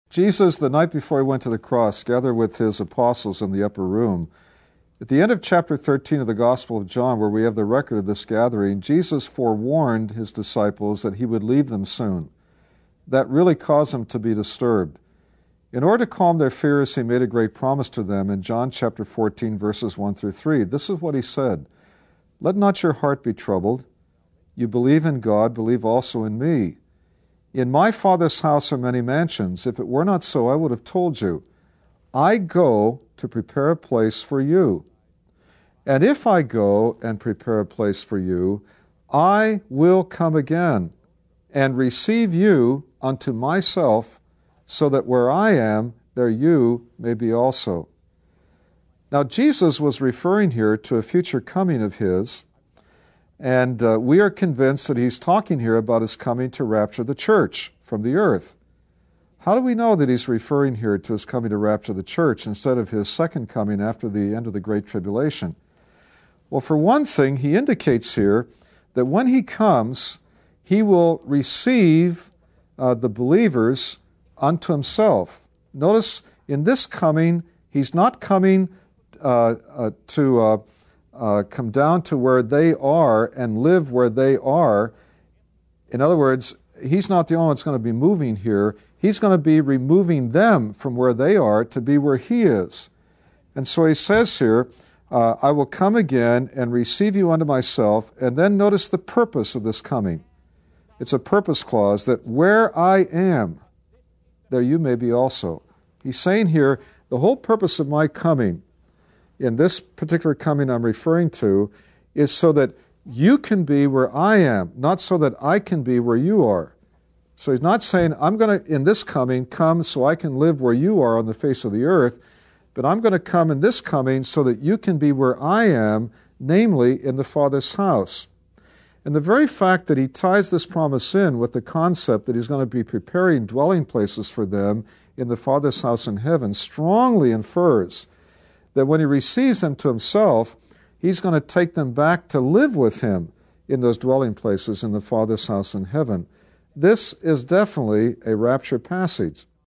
Why is John 14:1-3 a rapture passage?
This clip is from our series "35 most asked questions concerning Bible Prophecy".  This clip answers the question: Why is John 14:1-3 a rapture passage?